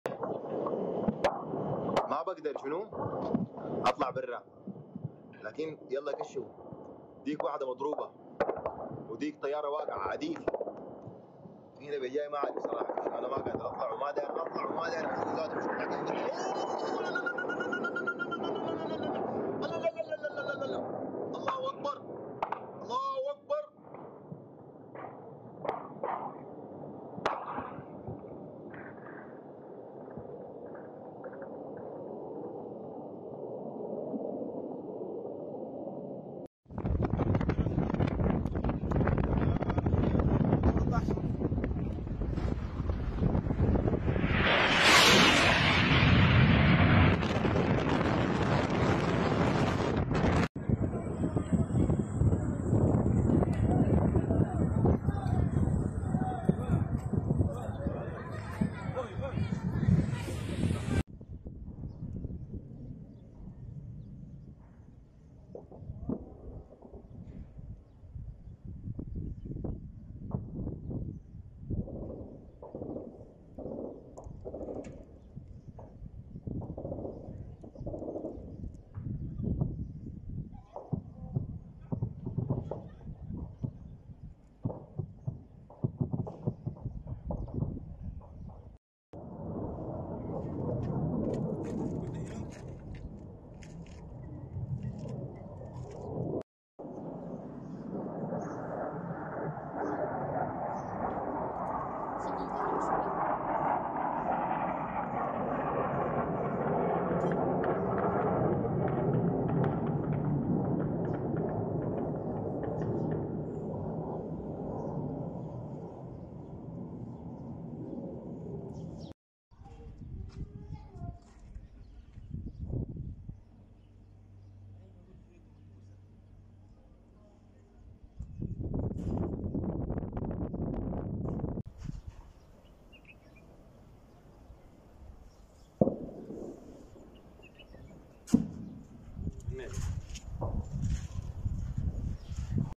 Це військовий переворот? ВІДЕО запеклих боїв у СУДАНІ
У столиці Судану та інших частинах африканської країни спалахнули запеклі зіткнення між збройними силами Судану та потужними воєнізованими формуваннями країни, що викликало побоювання щодо ширшого конфлікту в охопленій хаосом країні.

У Хартумі звук інтенсивної стрілянини було чутно в ряді районів.